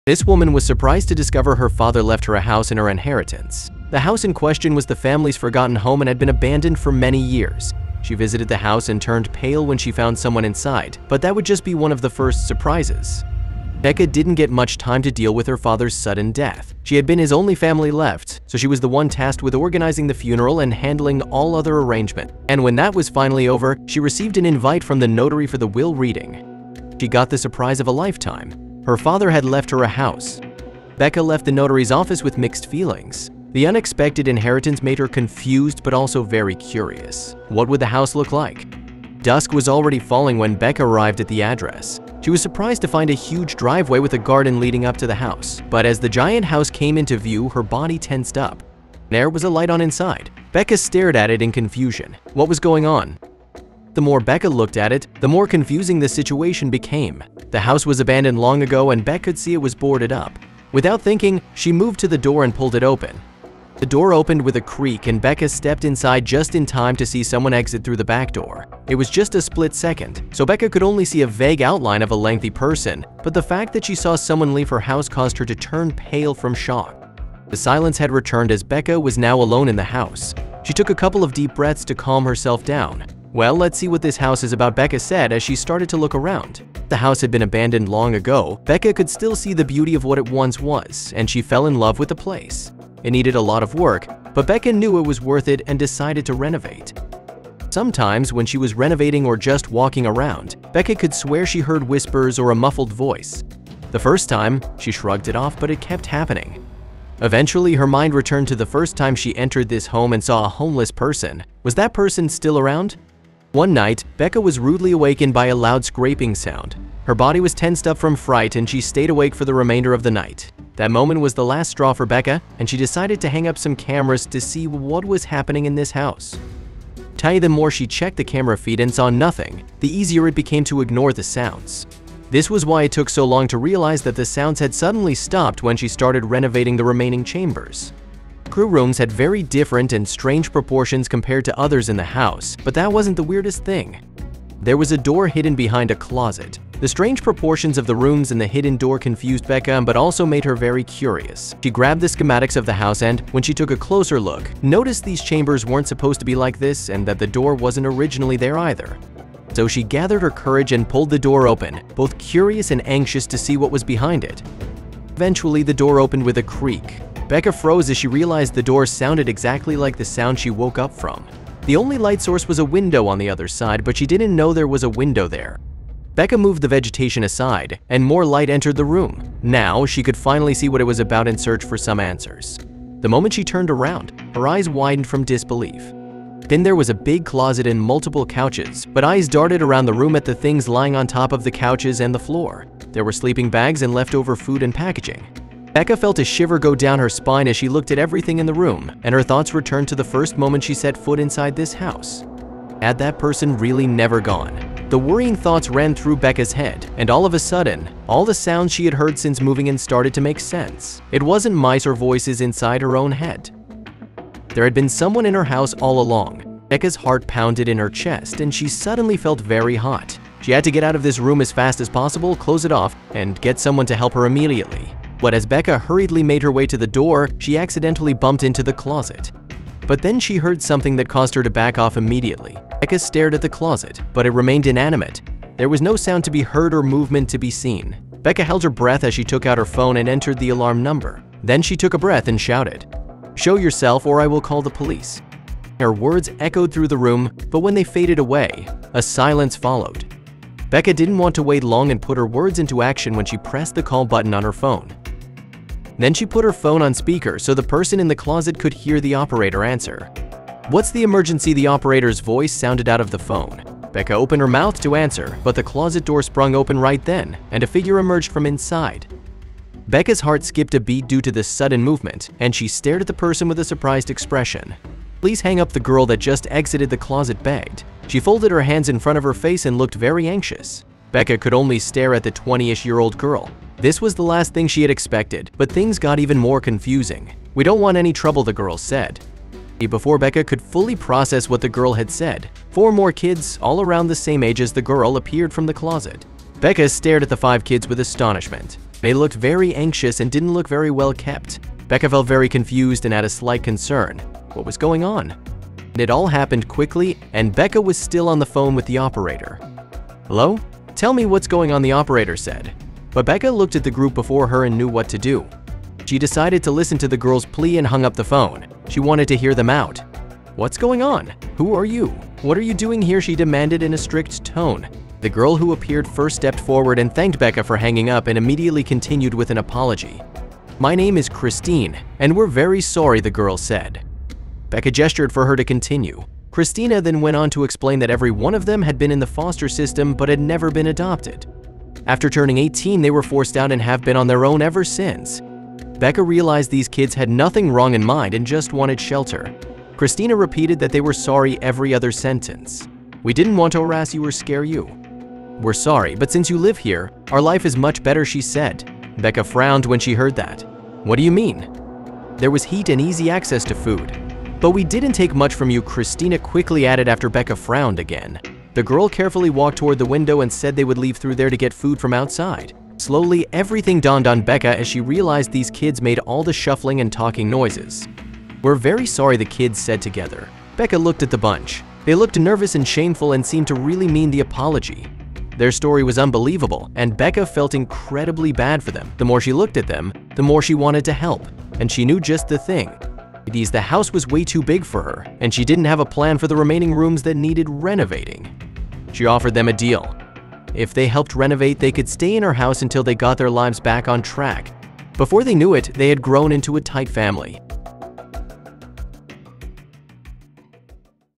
Woman Visits Abandoned Family Home, Turns Pale When She Goes Inside
This woman was surprised to discover her father left her a house in her inheritance. The house in question was the family’s forgotten home and had been abandoned for many years.